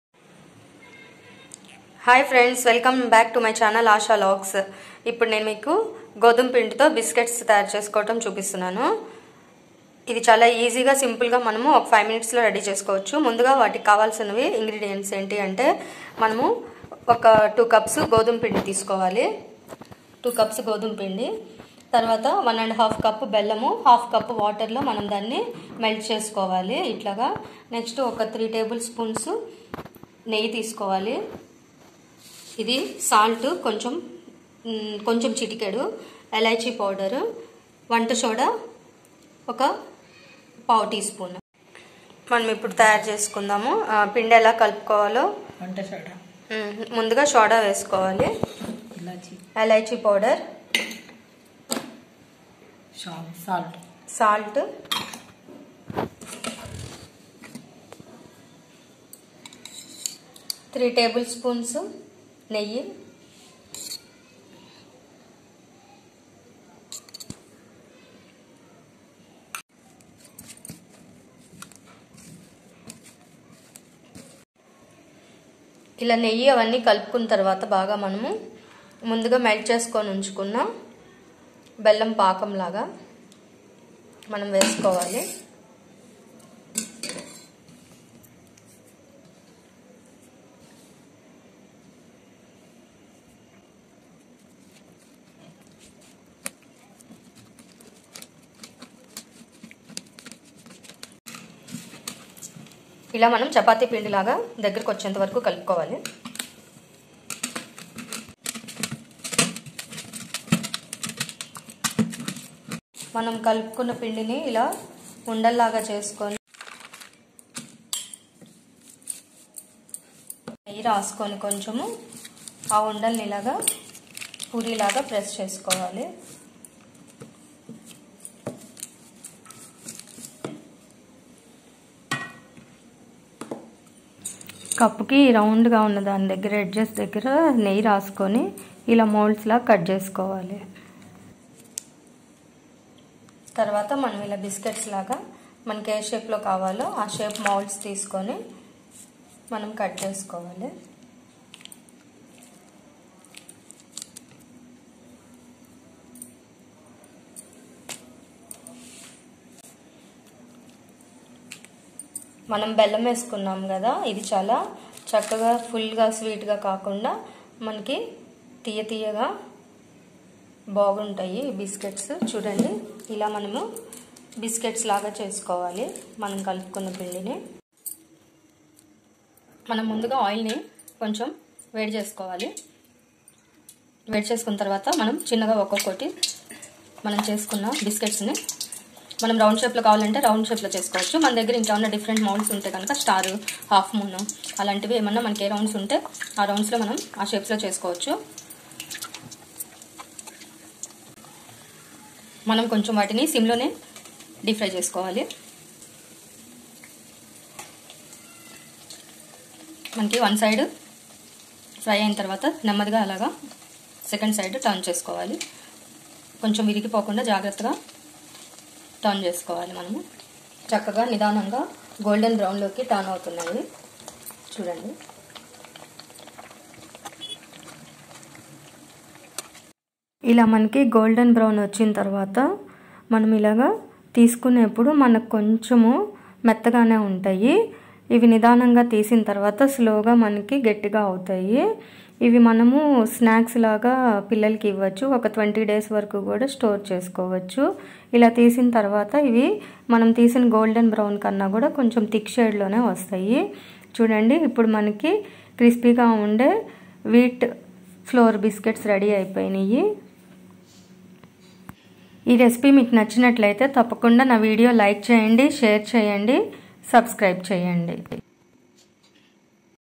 wheat flour biscuits || గోధుమపిండి బెల్లం బిస్కెట్స్ || Atta biscuit recipe in Telugu
Ingredients:
Wheat flour
Jaggery
ghee
elaichi powder
salt
soda

      Indian style whole wheat eggless biscuits have a rich and pleasant sweet taste and melt in the mouth.
      These atta biscuits might seem to be soft when frying but rest assured that they will get crispy dark brown colour upon cooling.